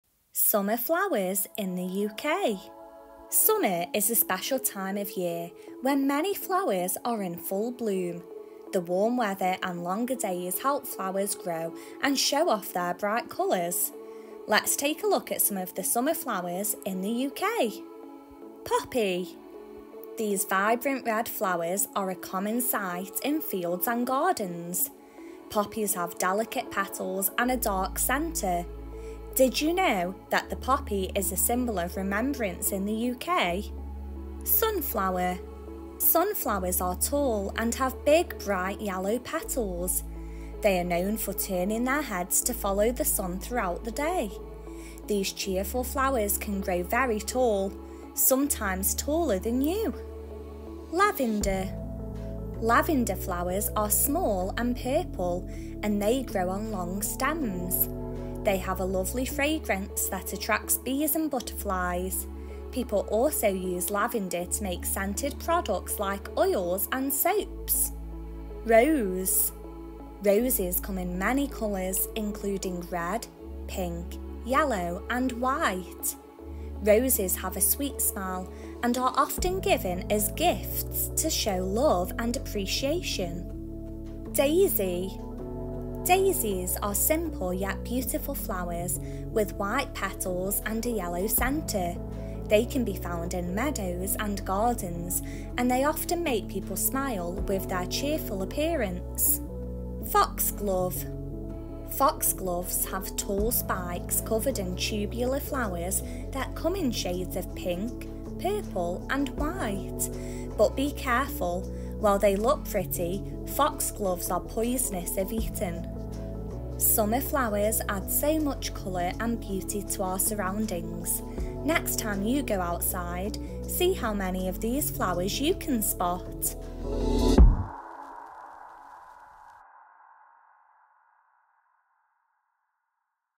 Summer Flower Identification for Kids! | British Summer Flowers
Learn all about summer flowers in the UK! 🌺🌻🌼

This video will help teachers introduce children to the beauty of summer flowers in the UK, including poppies, sunflowers, lavender, roses, daisies, and foxgloves. 🌸🌻🌹

What is summer?

Summer is a special time of year when many flowers are in full bloom, showcasing their bright colours in the warm weather and longer days. 🌞🌺

What are some flowers that bloom in summer in the UK?

Poppy
Sunflower
Lavender
Rose
Daisy
Foxglove

Sign up to Twinkl for FREE.

00:00 - What are some common summer flowers in the UK?
00:20 - What is the significance of poppies in the UK?
00:35 - How do sunflowers follow the sun throughout the day?
00:51 - What are some uses of lavender besides its fragrance?
01:09 - Why are roses often given as gifts?
01:25 - Where can daisies be found?
01:40 - What should you be aware of when it comes to foxgloves?